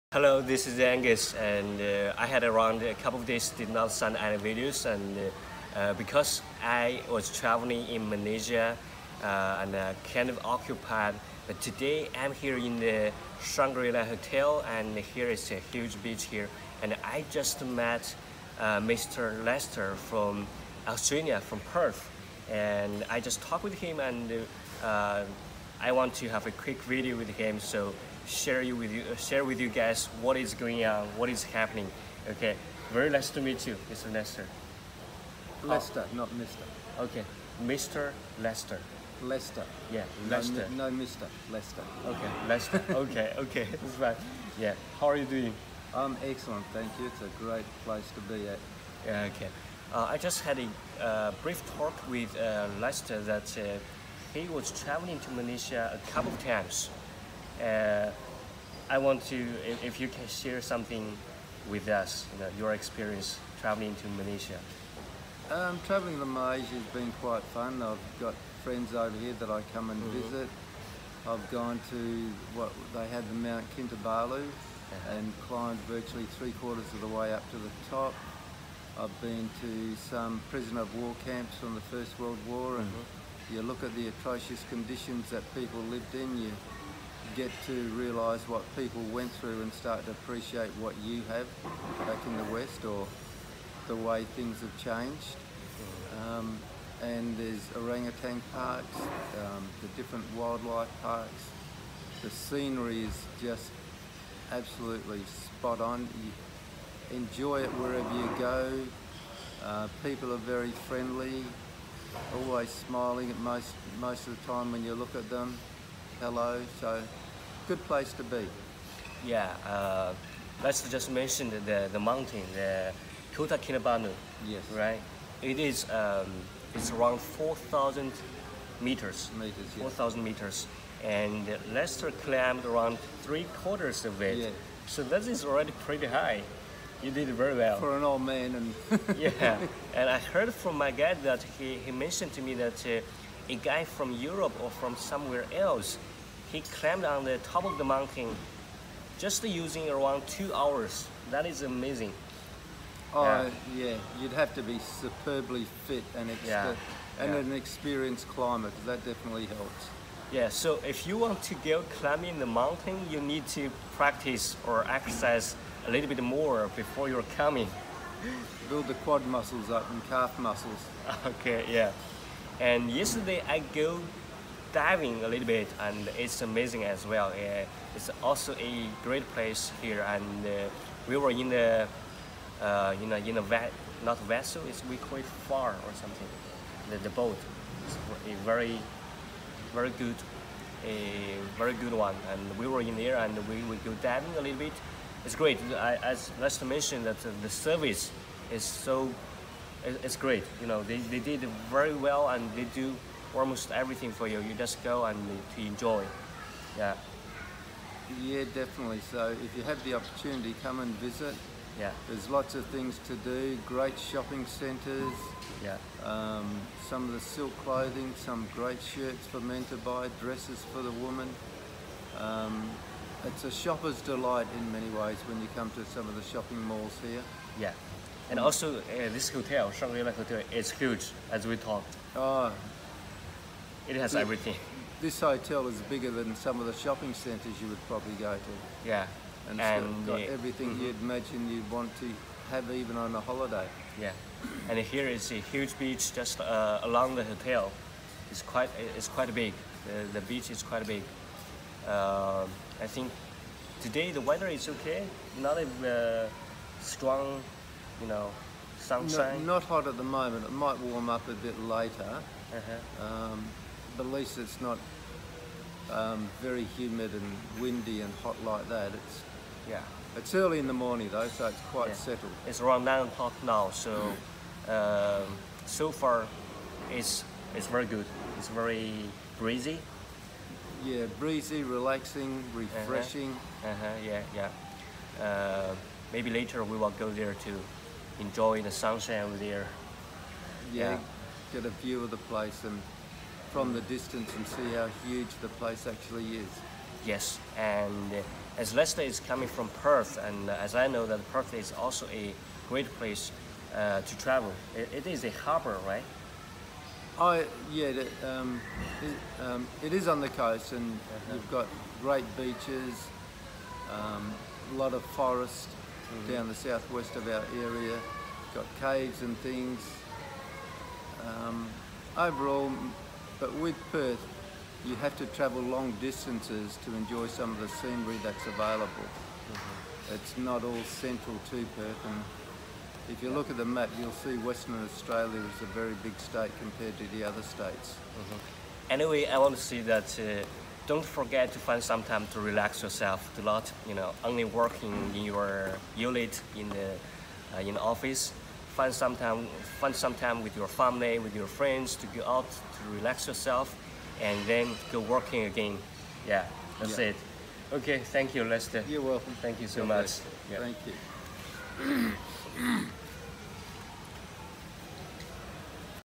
Talking with a Traveler in Kota Kinabalu (2018)
Attention: This video may not help you on anything. Just want to share.

Traveled to Kota Kinabalu for few days. Enjoyed a lot there. Though I did not have too much experience to share the beautiful scenery there and I was actually not feeling very well there because of the hot weather. I missed my YouTube friends. I want to share you things desperately. I got up in the early morning and met a Traveler there in the hotel. Had a few minutes talking with him.

For me, I think travel sometimes is not only about how beautiful the scenery it is, how many great pictures you took, it is also about if you met with people or come across new ideas you did not know before.